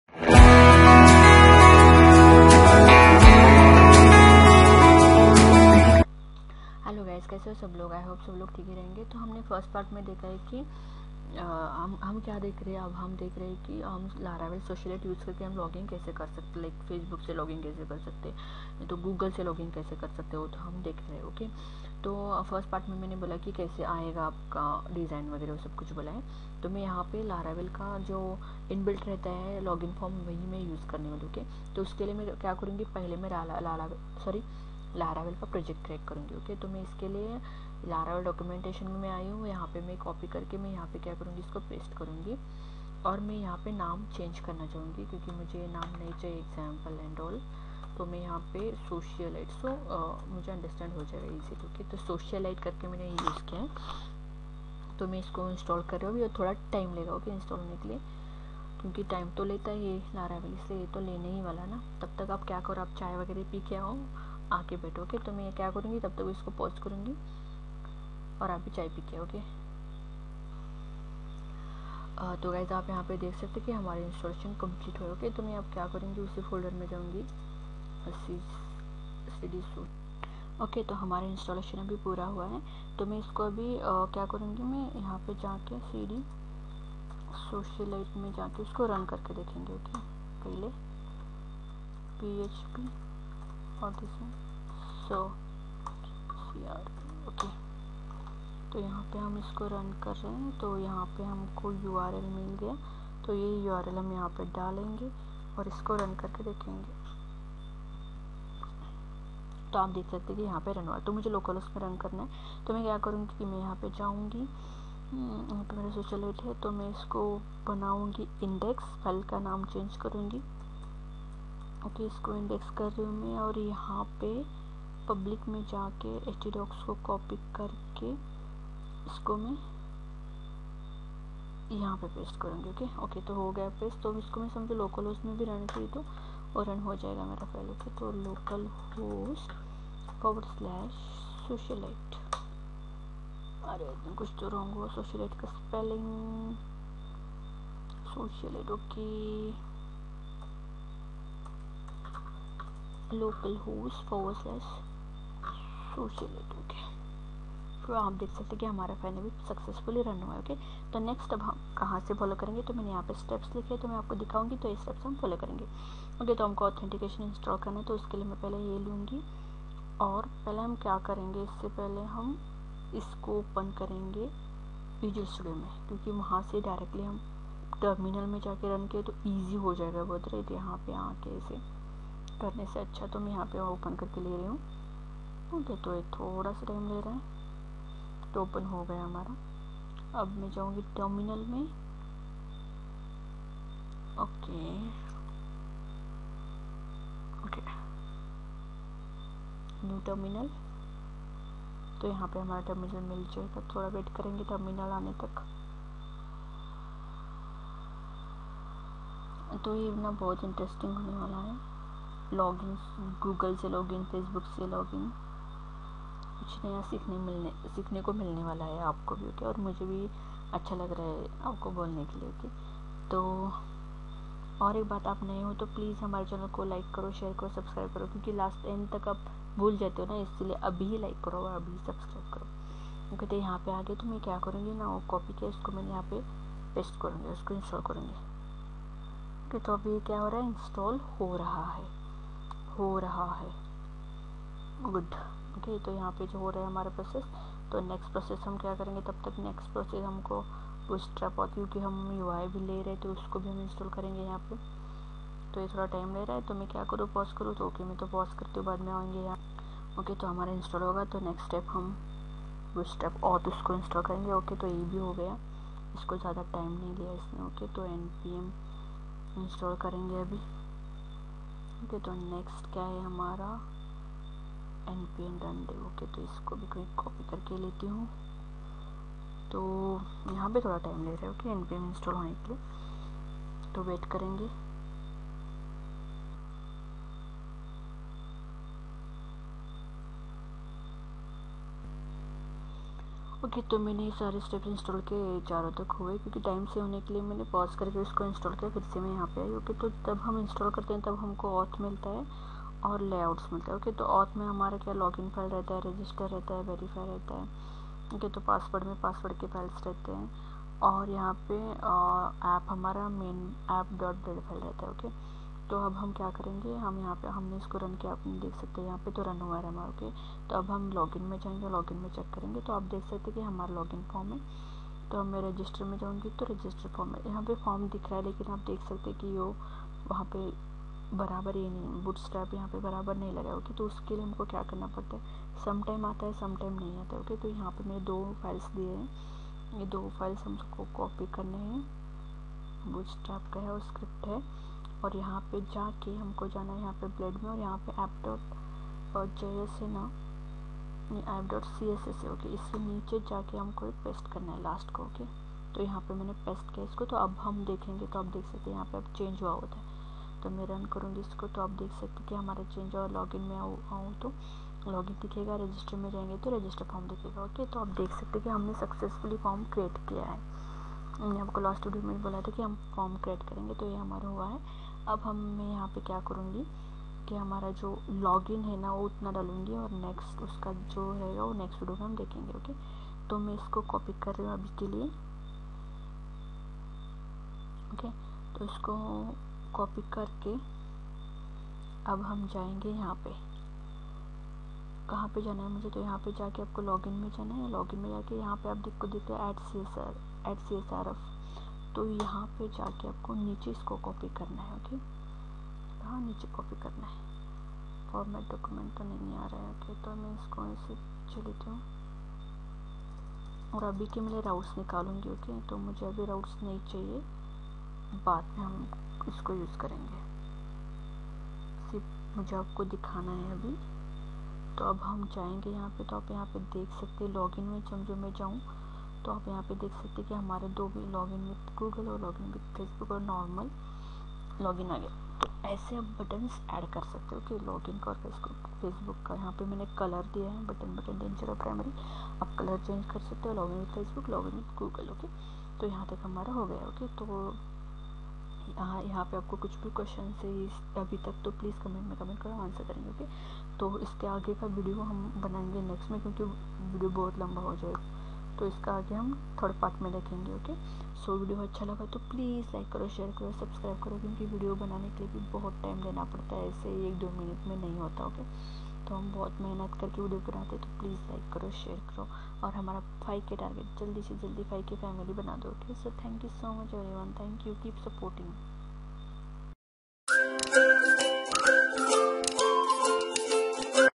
How to set login with Google in laravel? Laravel Socialite Login with Google and Facebook [PART-2]
How to set login with Google in laravel? Laravel Socialite Login with Google and Facebook [PART-1]

How to set login with Google in laravel?
How to use Google API in Laravel 8?
How to log in with OTP in Laravel 8?
How to create user authentication in Laravel 8?